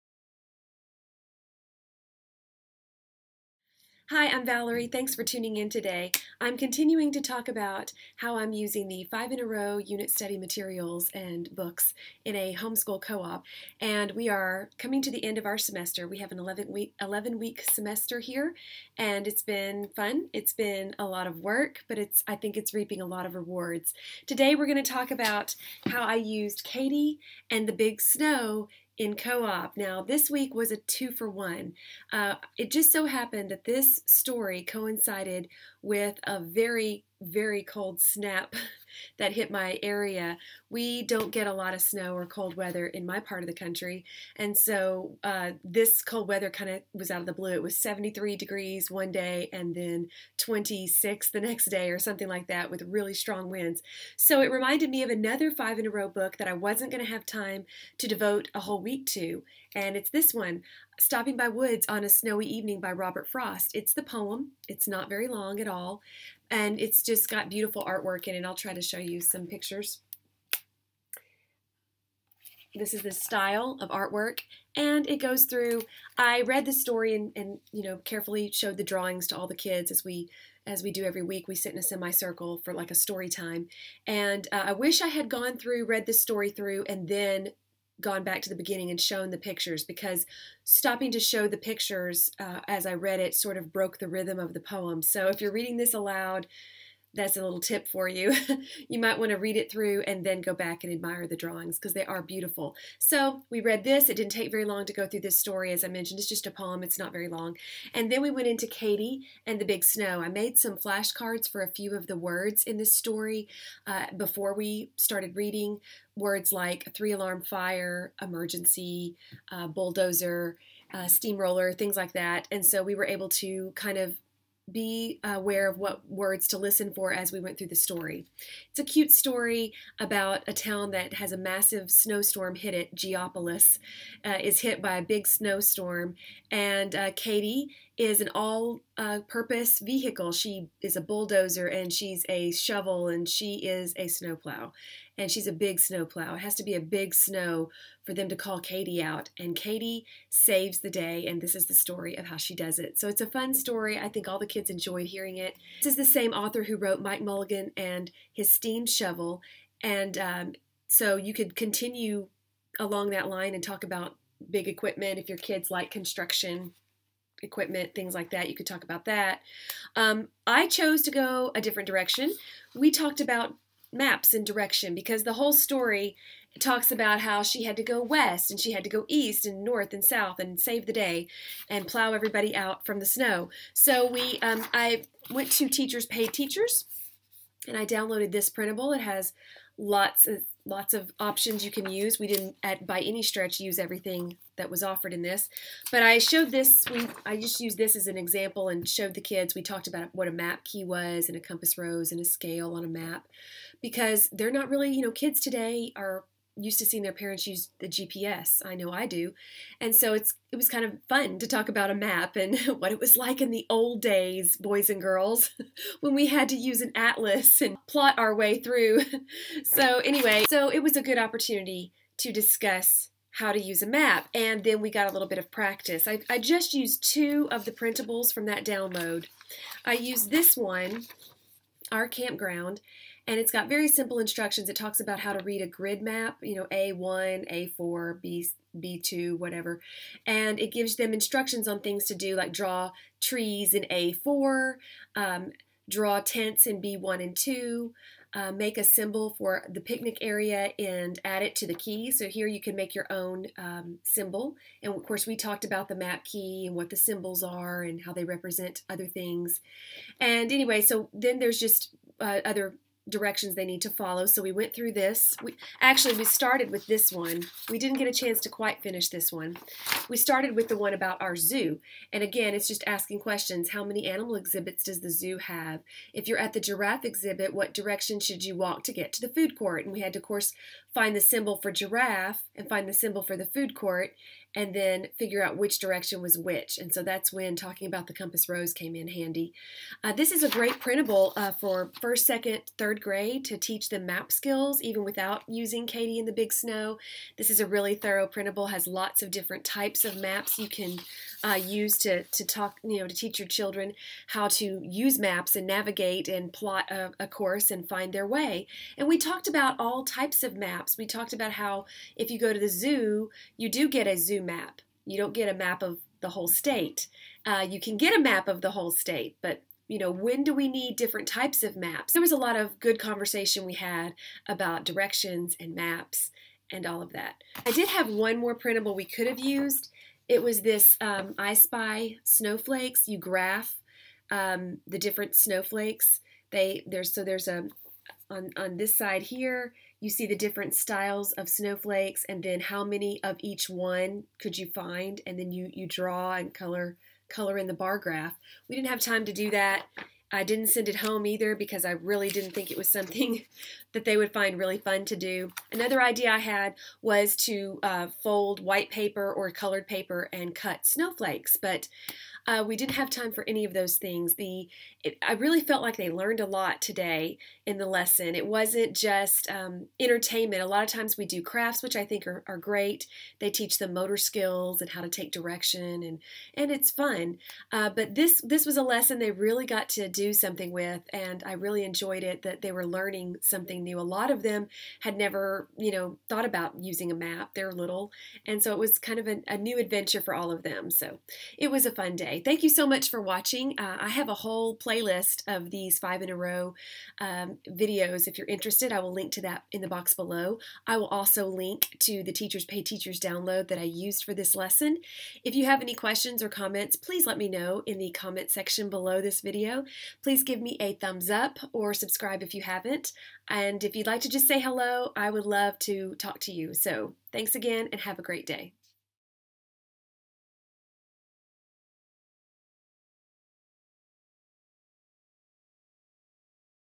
How to Use Katy and the Big Snow in Co-Op
Here's what we did in co-op with the book Katy and the Big Snow!